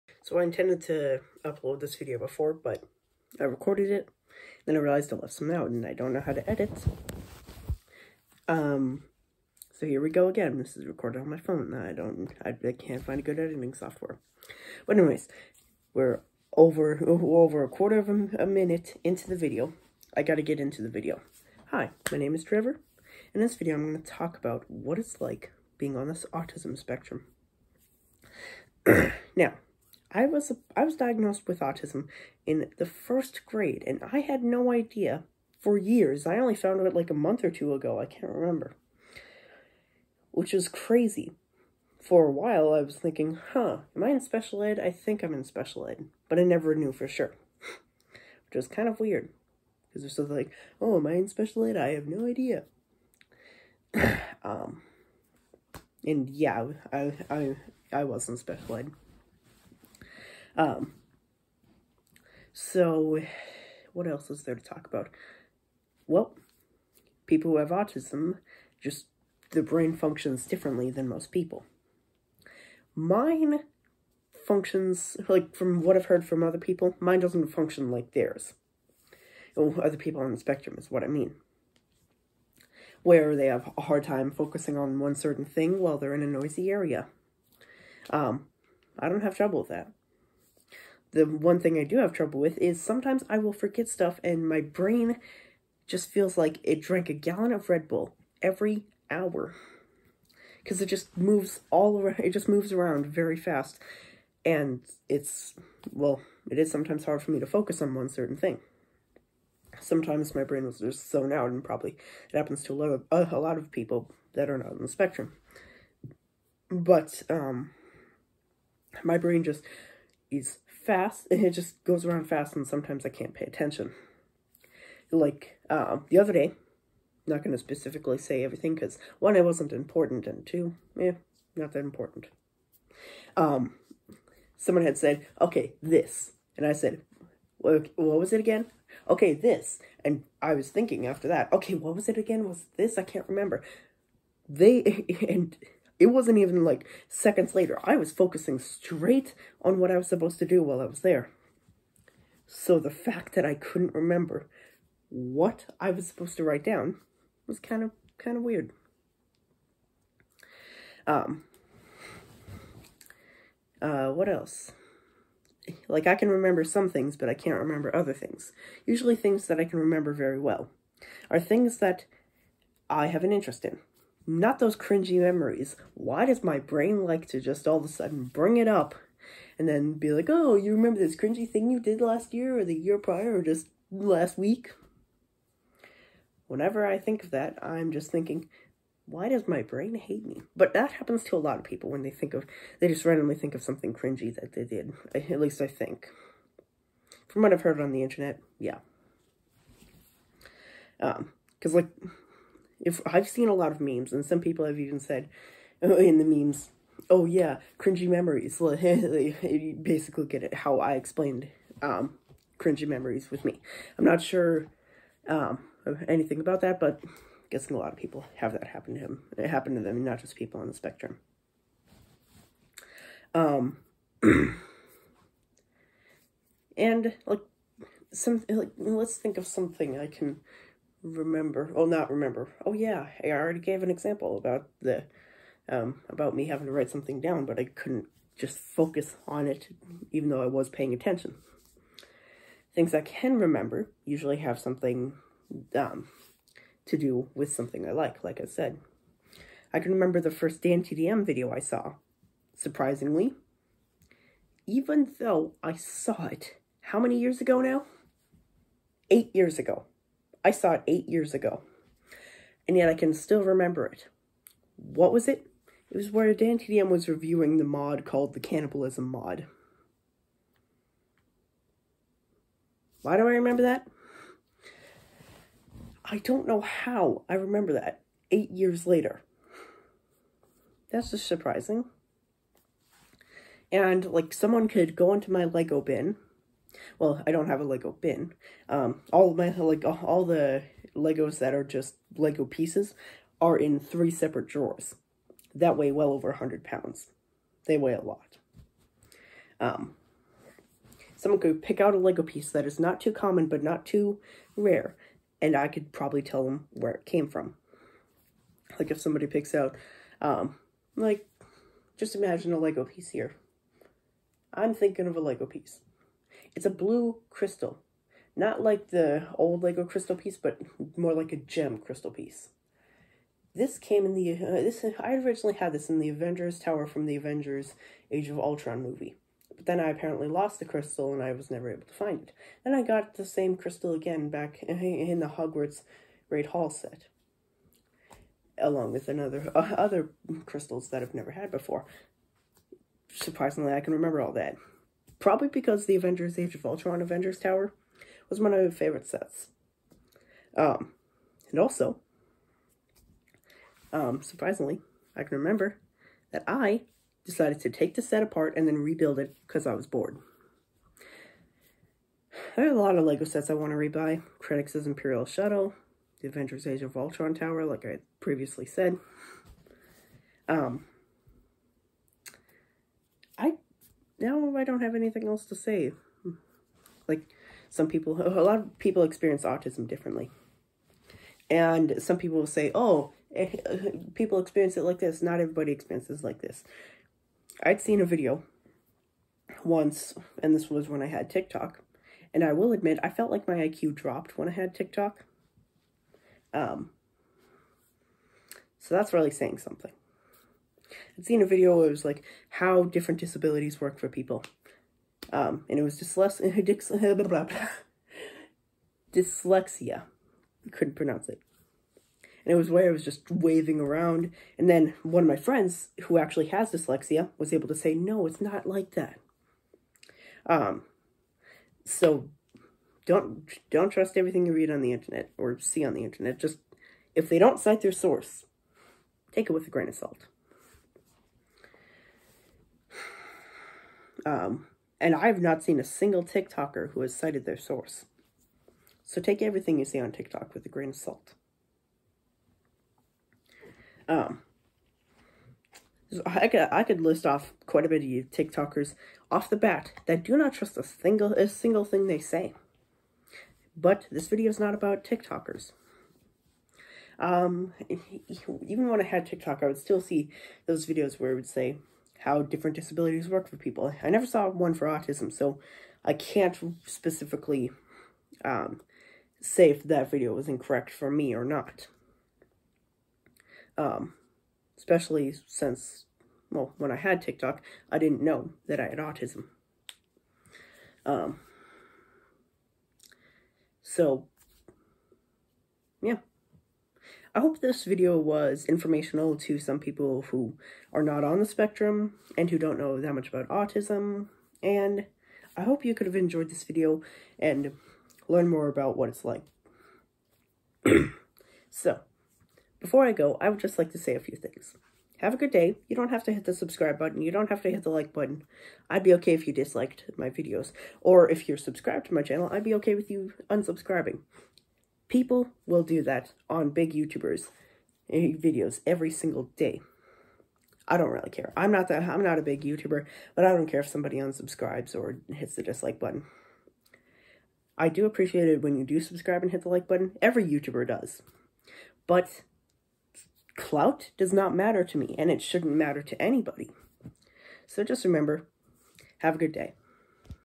What it’s like being on the spectrum
For a bit I was upset to think about being autistic but I learned to except it.